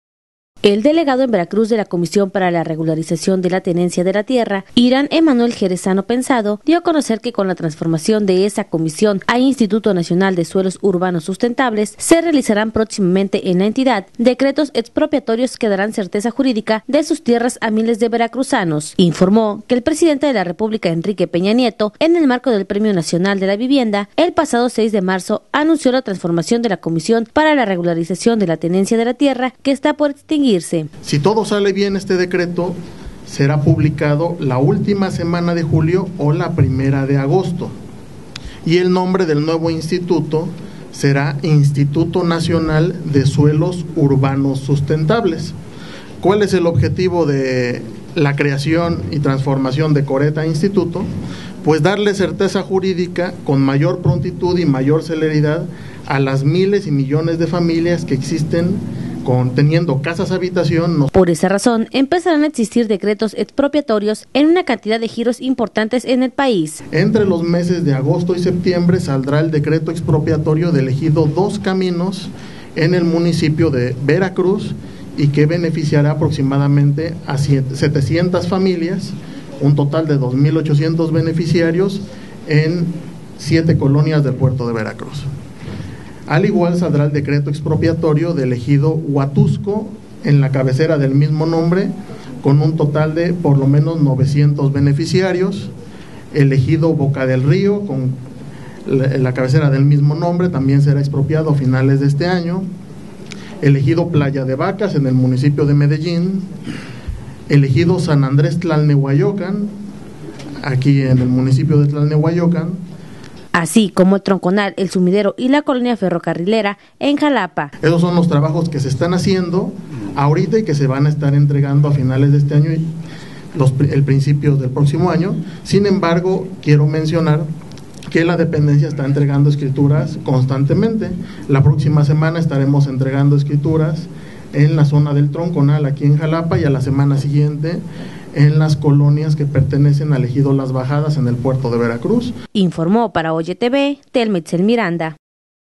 CORETT SE TRANSFORMA EN INSTITUTO NACIONAL DE SUELOS URBANOS SUSTENTABLES
El delegado en Veracruz de la Comisión para la Regularización de la Tenencia de la Tierra (Corett), Hiram Emmanuel Jerezano Pensado dio a conocer que con la transformación de esa comisión a Instituto Nacional de Suelos Urbanos Sustentables, se realizarán próximamente en la entidad decretos expropiatorios que darán certeza jurídica de sus tierras a miles de veracruzanos. Informó que el presidente de la República,  Enrique Peña Nieto en el marco del Premio Nacional de la Vivienda, el pasado 6 de marzo anunció la transformación de la Comisión para la Regularización de la Tenencia de la Tierra que está por extinguirse.